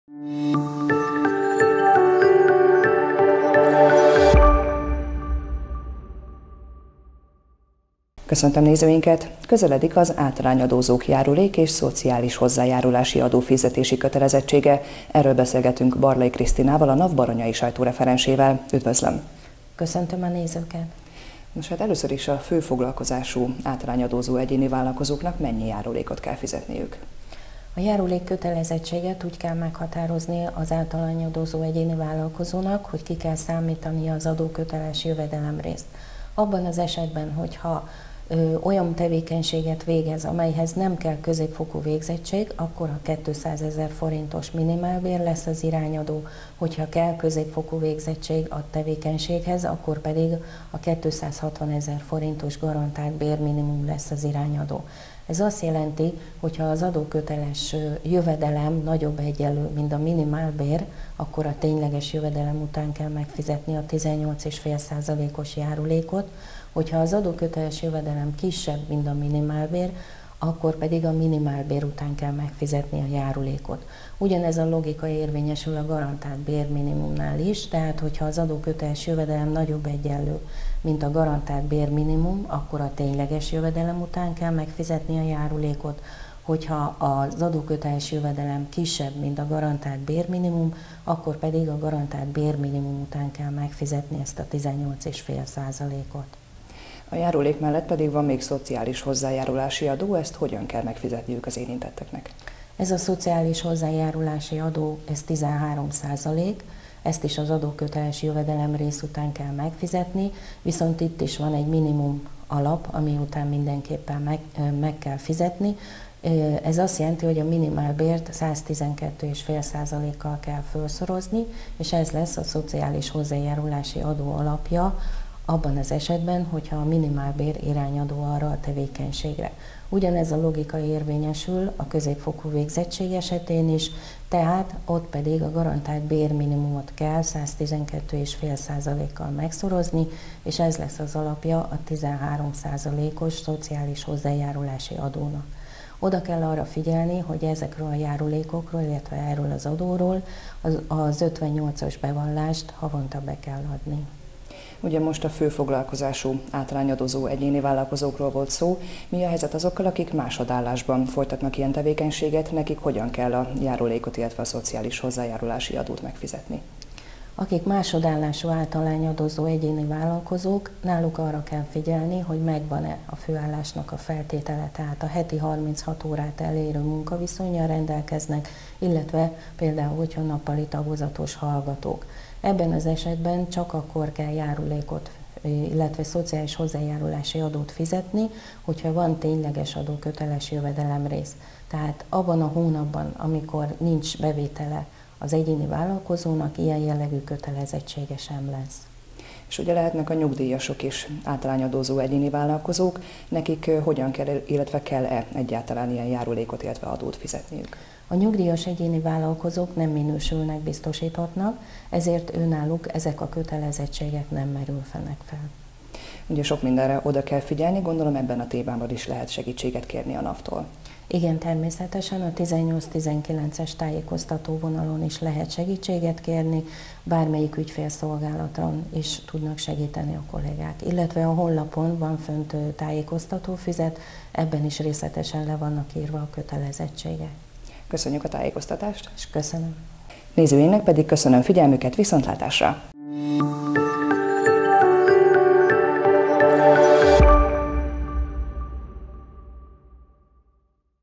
Adópercek
Téma: Az átalányadózóknak járulékot és szociális hozzájárulási adót is kell fizetniük minden hónapban. Kötelezettségüket havonta be is kell vallaniuk, a befizetendő összeget pedig jövedelmük alapján határozzák meg. Más szabályok vonatkoznak azonban a fő- és másodállású, valamint a nyugdíjas átalányadózókra is.